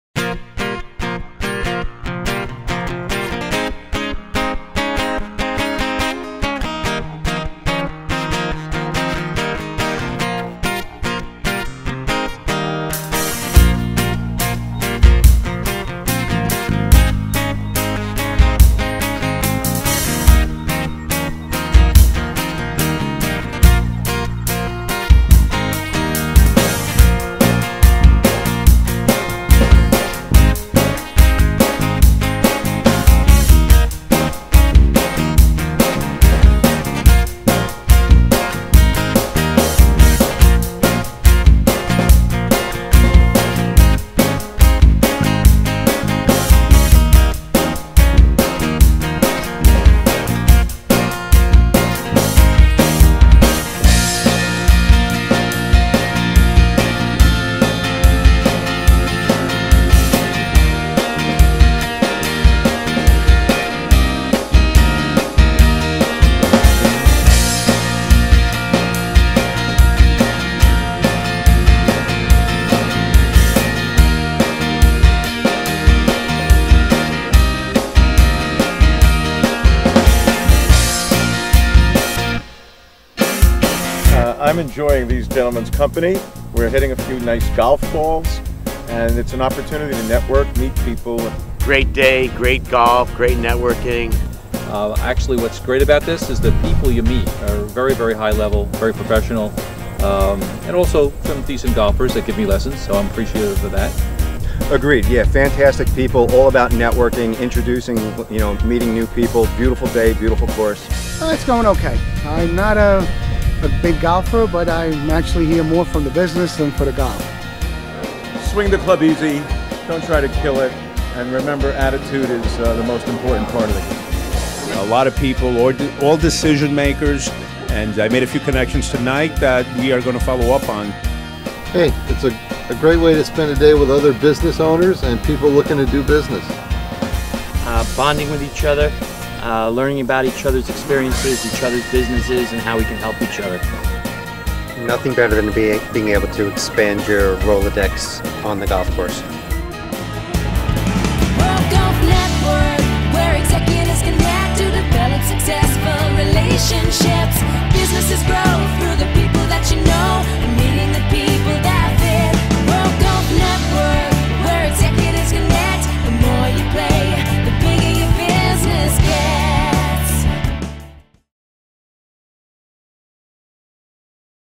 World Golf Network 2015 Highlights by HTV
Great business opportunities happen here at the World Golf Network.  Executive  and key business decision makers meet over golf - and make business happen!